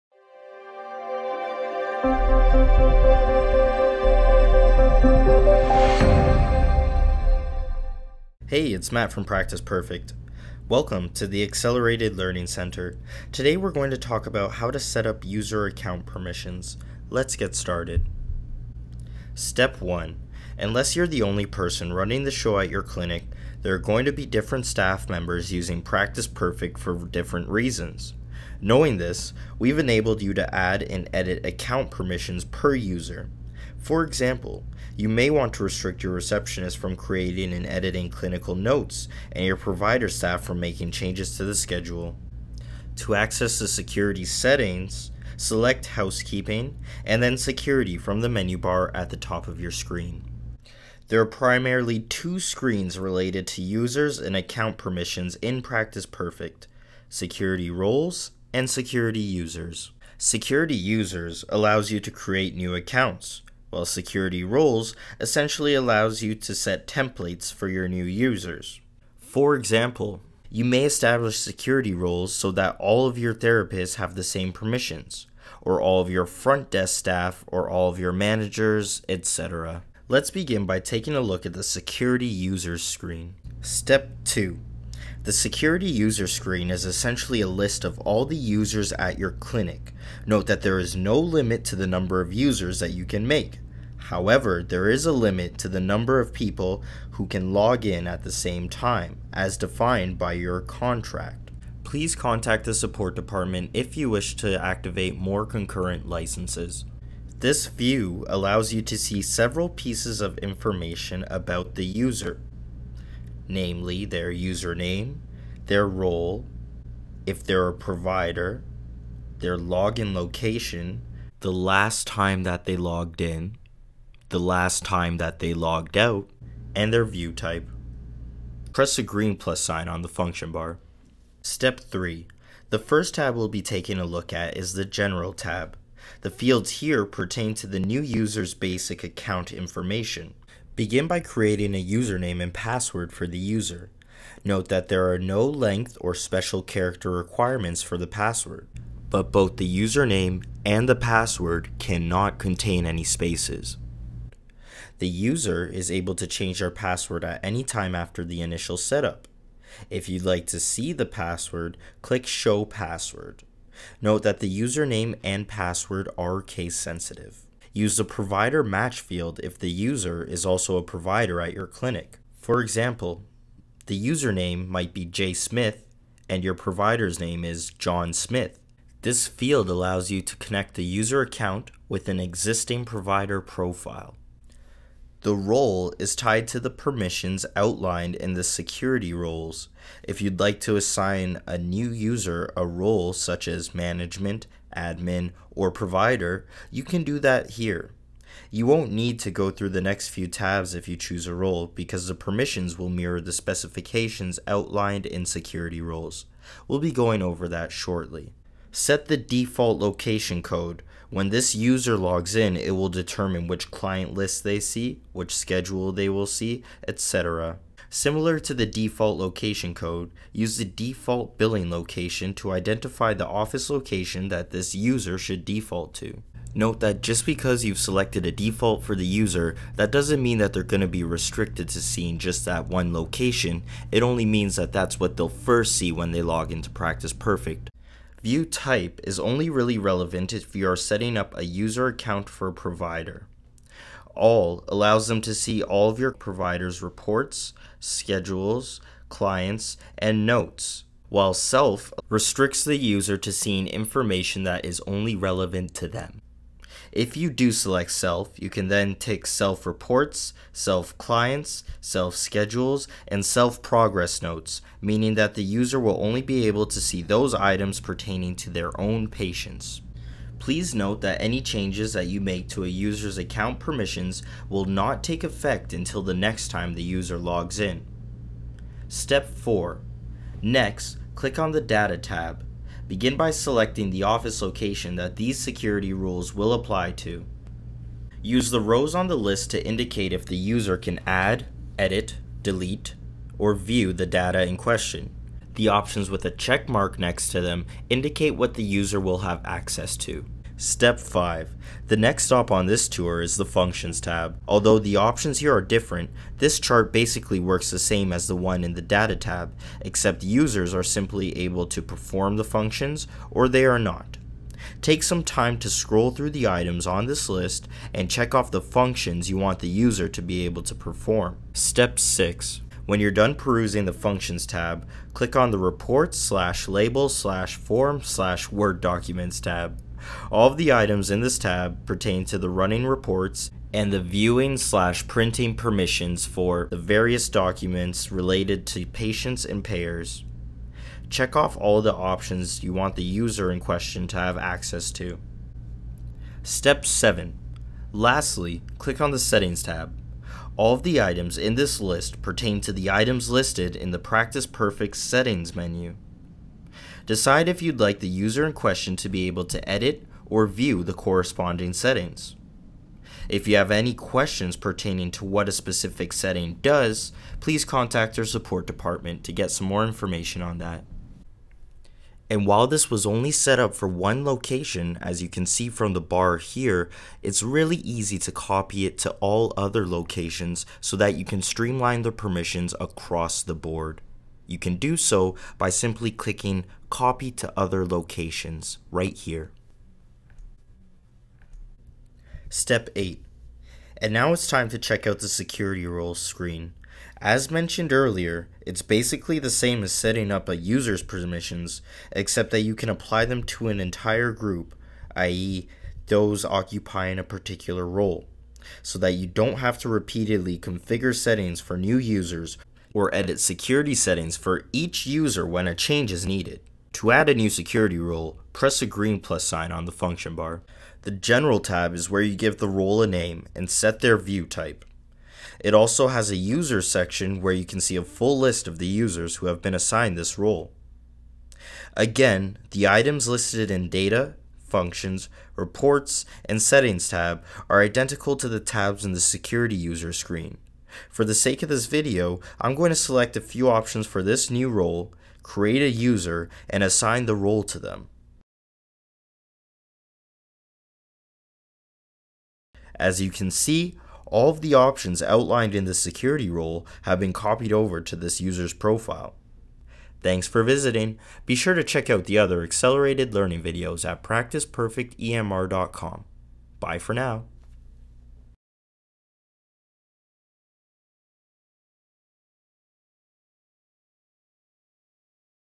How to setup new users and account permissions
This video discusses how to create new users and setup their account permissions in Practice Perfect. This is a key process that effectively gives your staff access to Practice Perfect.

If you require assistance with creating new users, or you wish to activate additional licenses, please